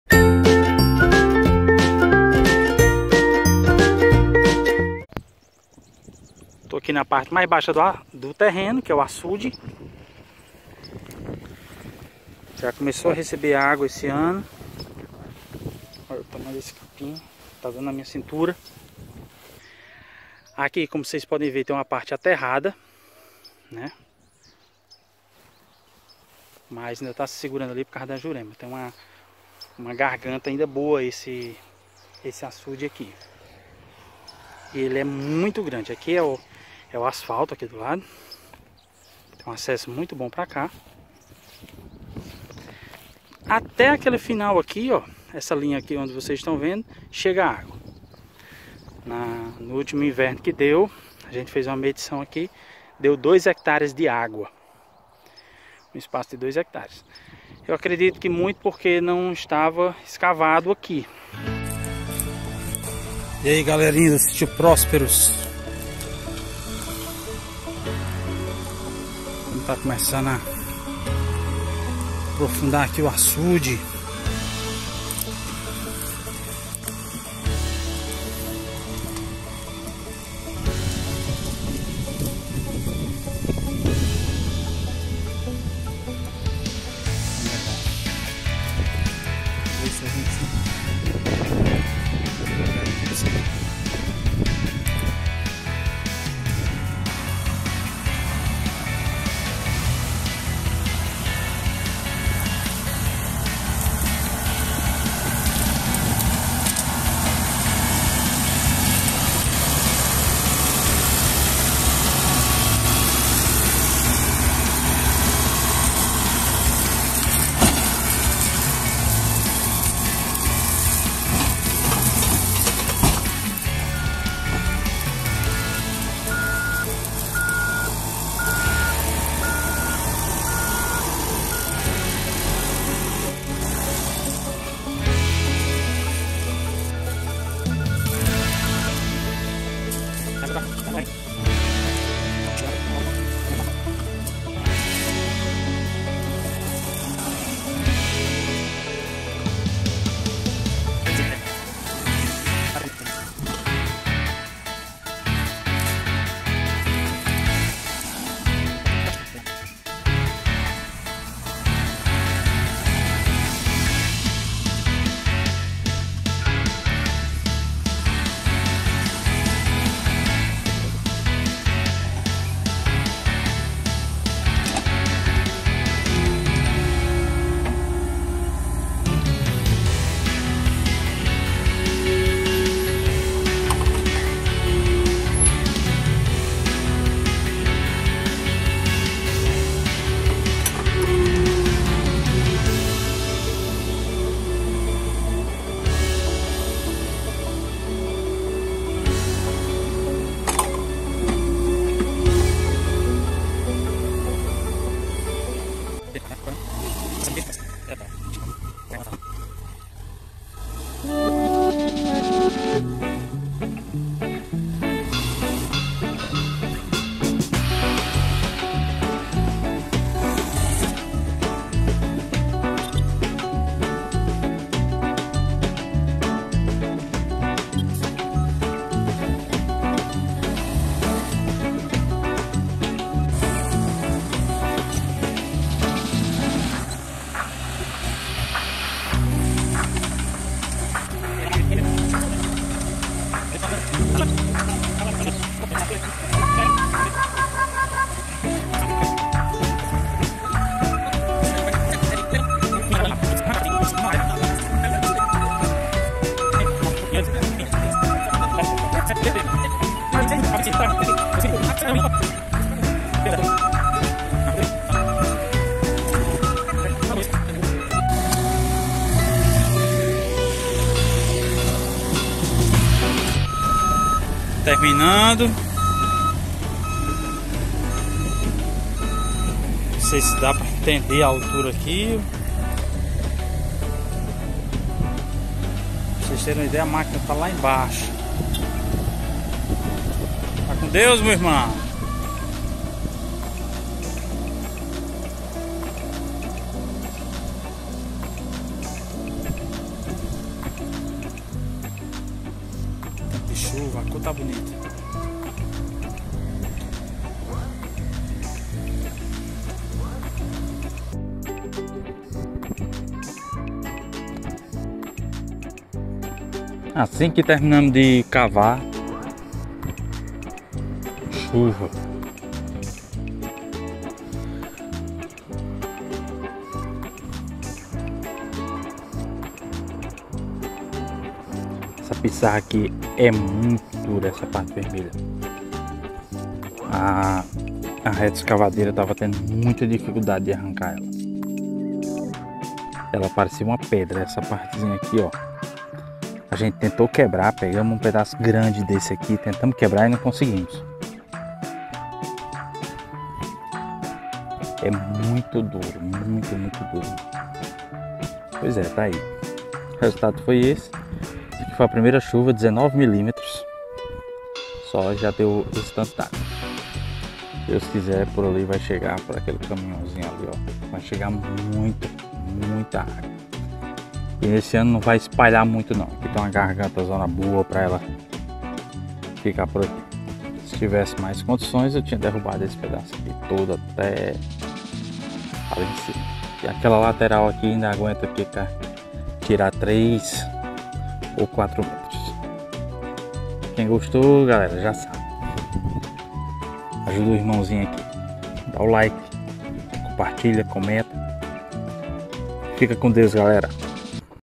LIMPEZA E APROFUNDAMENTO AÇUDE DO SÍTIO PRÓSPERUS [#72]
LIMPEZA E APROFUNDAMENTO AÇUDE DO SÍTIO PRÓSPERUS

"A limpeza e revitalização de açudes e barragens tem como objetivo melhorar os reservatórios de água para os agricultores. Entre os principais serviços está a reconstrução de paredes, reforço nas calhas de escoamento da água e a limpeza de um modo geral"